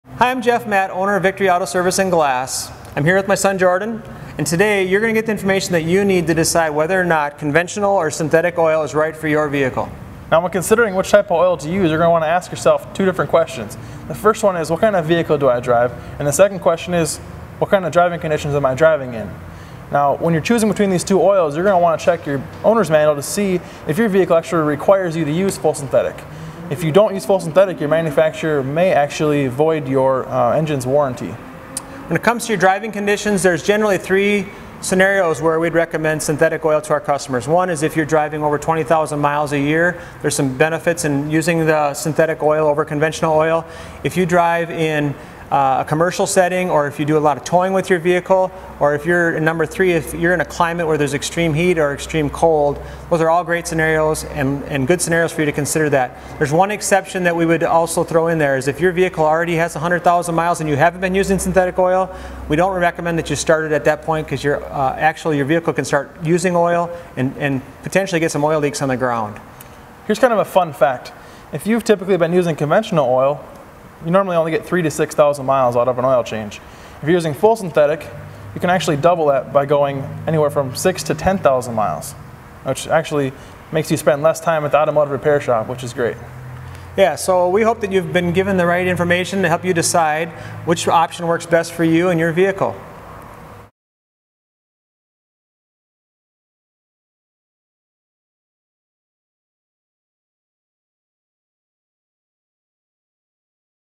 What's the Difference Between Conventional (Regular) and Synthetic Oil?
Wondering whether you should put regular or synthetic oil in your vehicle? Are you thinking, what's the big difference between them, other than synthetic costs more? Is synthetic oil one of those scams to get me to spend more? Great questions!

Some vehicles DO require synthetic oil, some vehicles do NOT, and some SHOULD NOT use synthetic oil. So, what about your vehicle? We've got you covered. In this short video, Jeff Matt and Jason Matt of Victory Auto Service & Glass in Minneapolis, MN, break down the main differences between the two in a super simple, easy-to-understand way.

Have a question for Jeff about your car? Post your question on the Victory Auto Facebook or check out the Victory Auto . We're here to help!